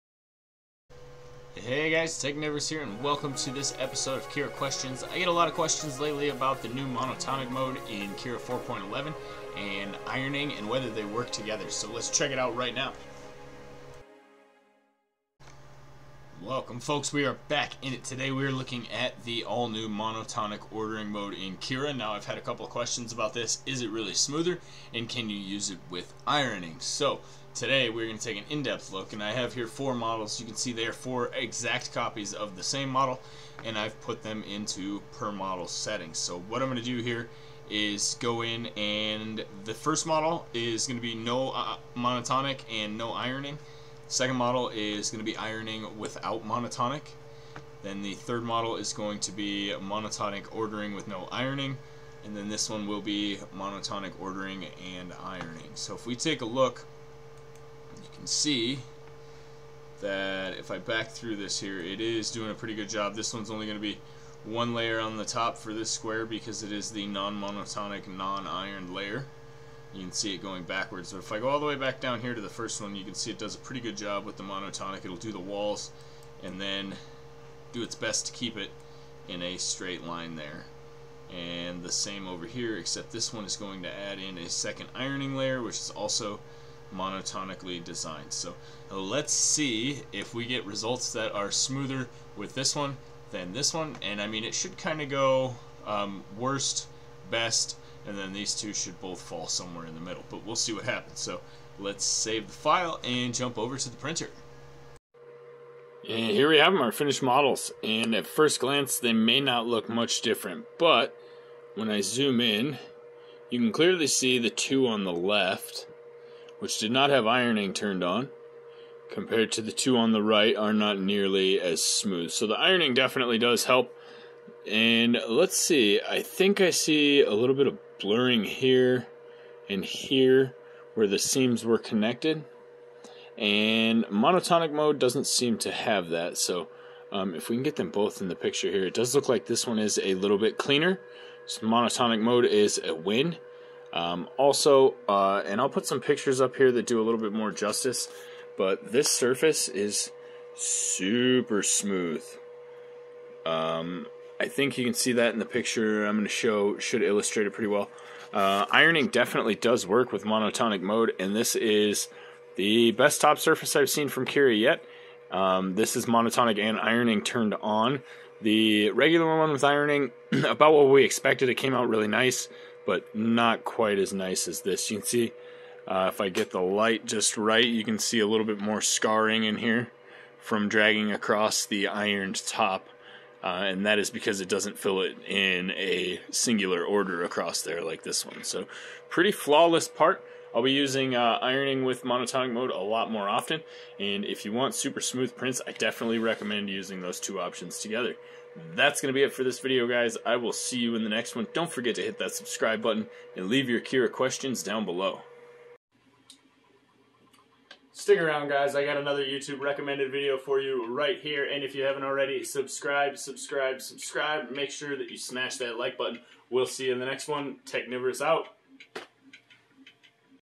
Smoother prints in Cura 4.11, with AND without ironing!
Technivorous shows you the newest feature of Cura 4.11, Monotonic Ordering. He compares it to standard slicing and then shows you how to get even smoother top surfaces on your 3D prints by using ironing WITH the new mode!

0x95F4EaB8A738F4F27e7c04aEe1E10C8E666C2D0c
31zvj8ybxVVTLw6nPvAHYqu3qDVhC1sRqk

SUPPORT TECHNIVOROUS 3D Printing:

become a channel member for youtube perks!

Technivorous
902 n Hughes